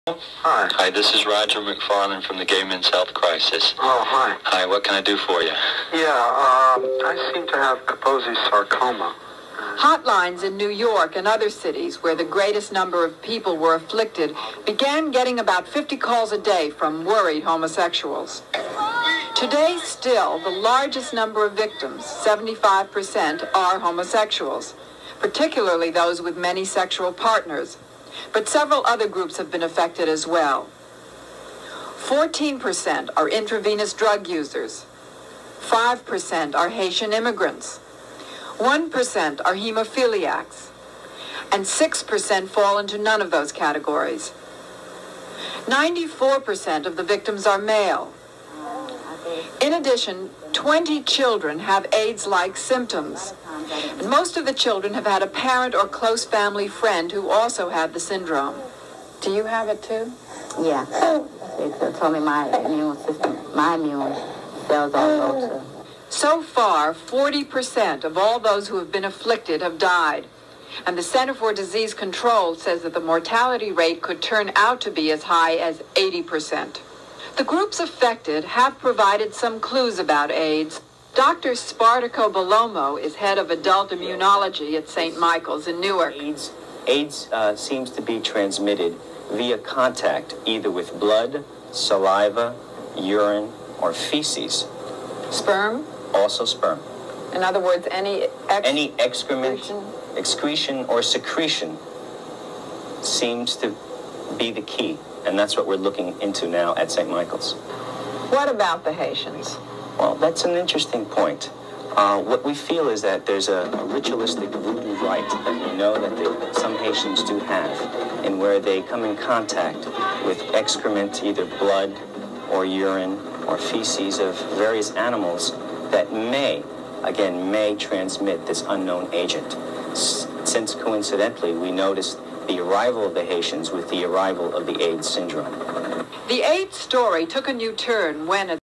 Early AIDS Experts On TV Saying AIDS Was Possibly Spread by Haitians Doing Voodoo Rituals
This is known as AIDS Think. Always wrong, always absurd, always racist, and always guessing. ON TV before millions of people…